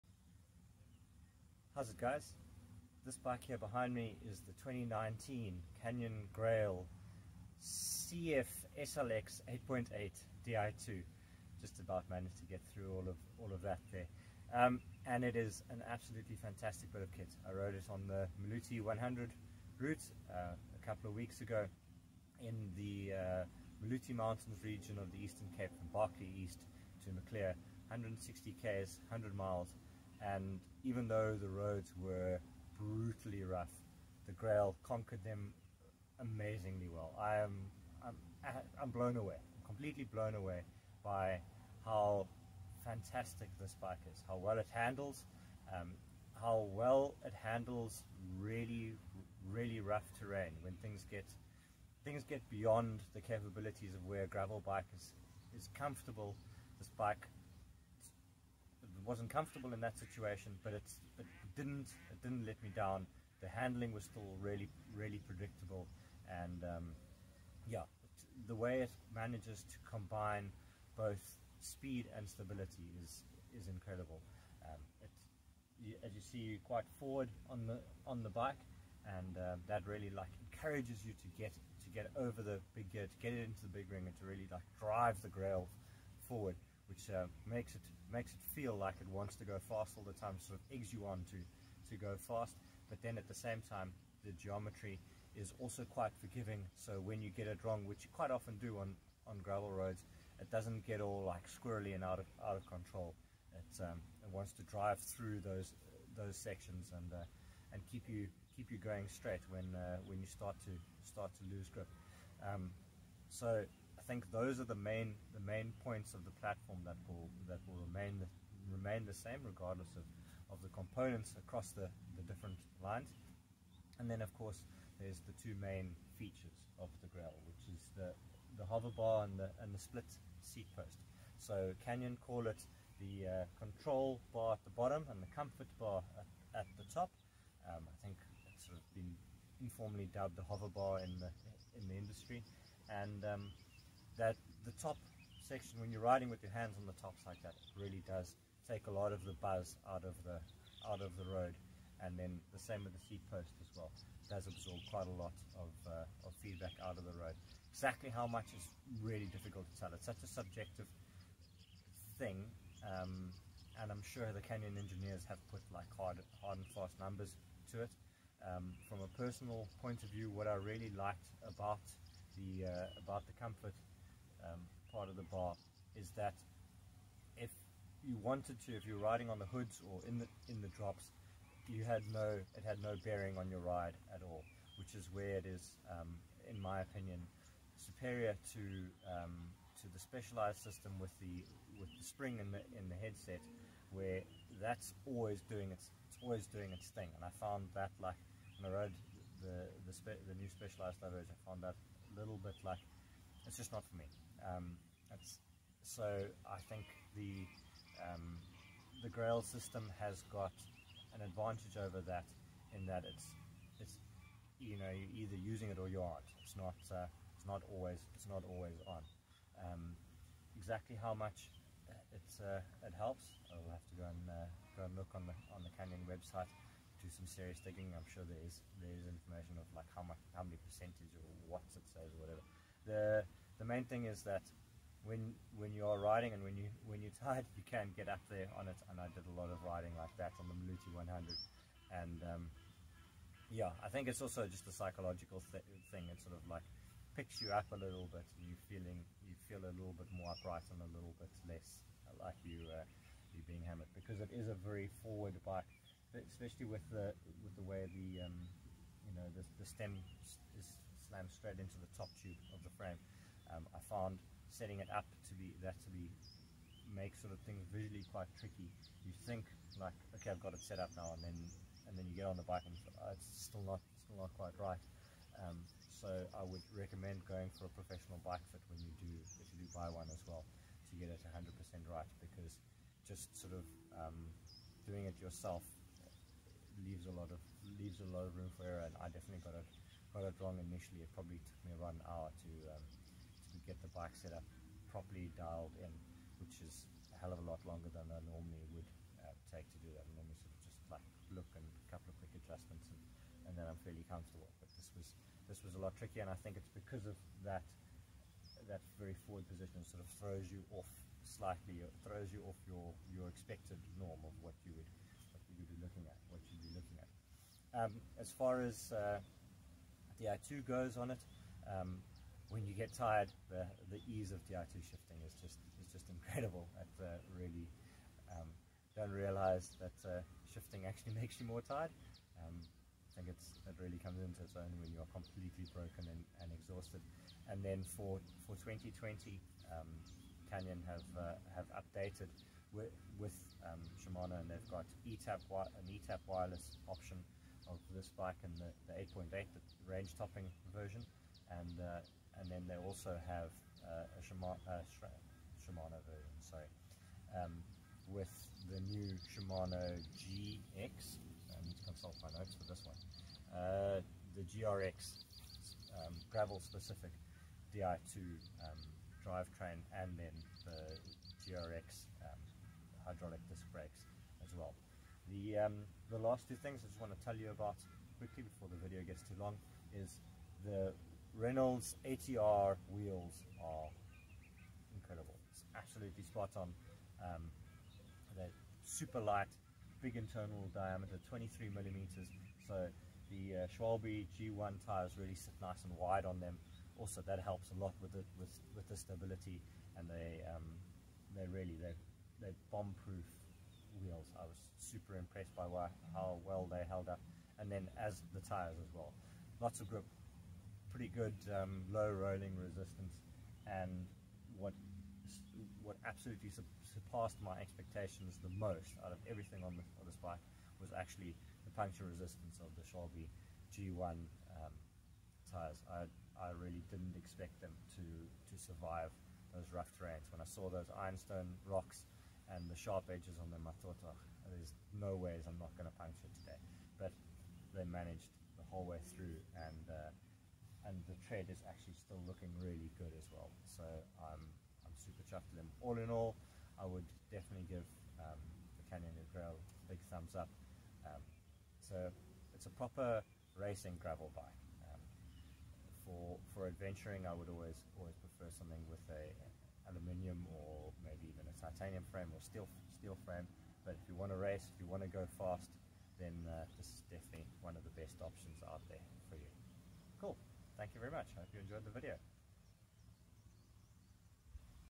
Canyon Grail CF SLX 8.0 Review
Take a look at what Seamus Allardice made of the Canyon Grail which he rode in the inaugural Maloti 100. It's an exceptionally impressive gravel racing machine. The Grail is remarkably stable, despite the aggressive position it eggs you into as a rider.